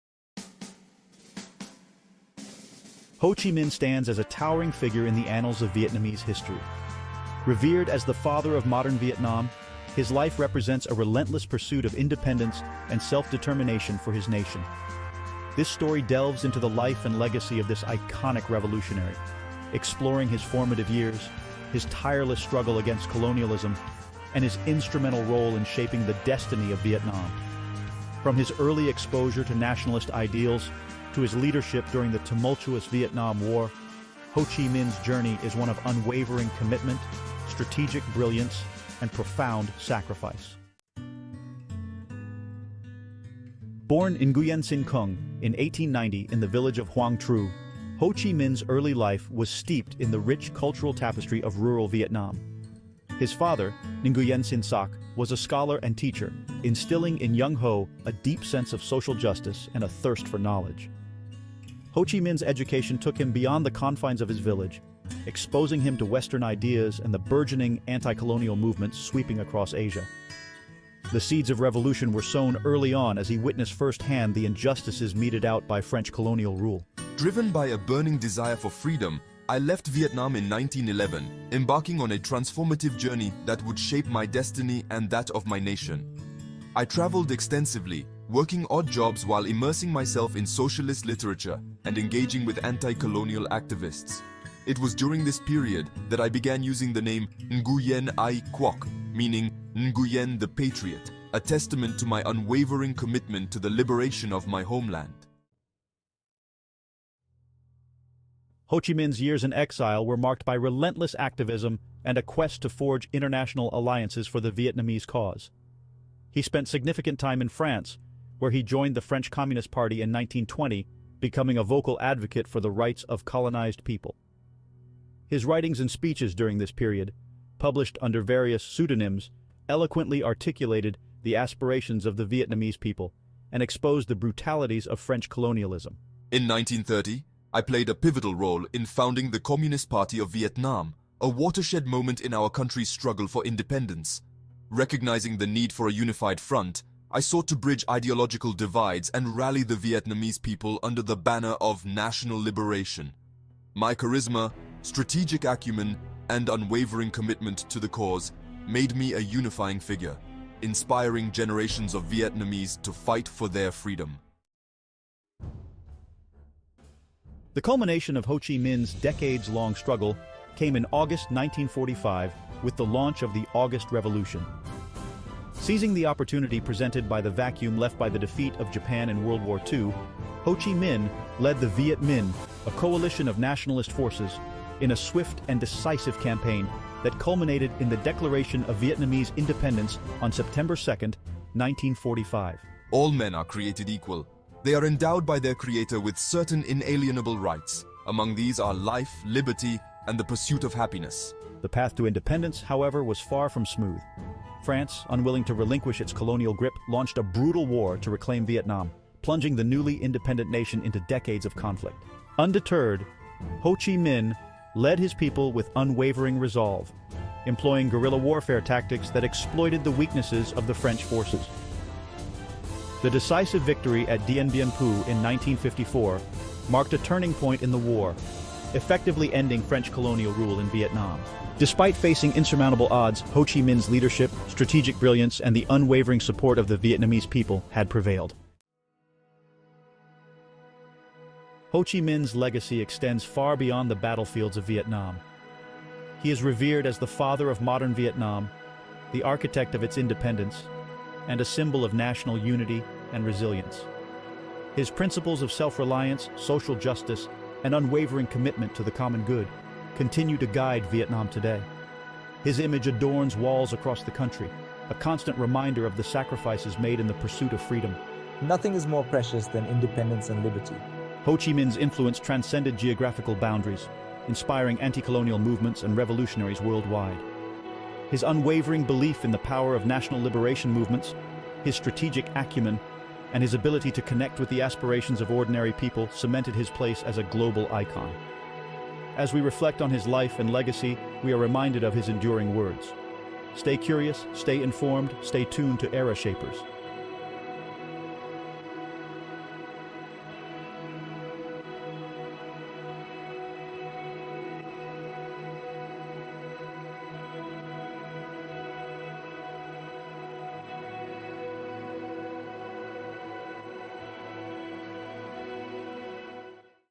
Ho Chi Minh - The Leader of North Vietnam
Timestamps:
00:00 A Beacon of Vietnamese Nationalism
00:40 The Making of a Revolutionary
01:45 Sowing the Seeds of Revolution
02:44 The August Revolution and Beyond
03:58 The Father of Modern Vietnam

End-Screen: Video by 🧪motionlabry - Motion Graphics Production from Pixabay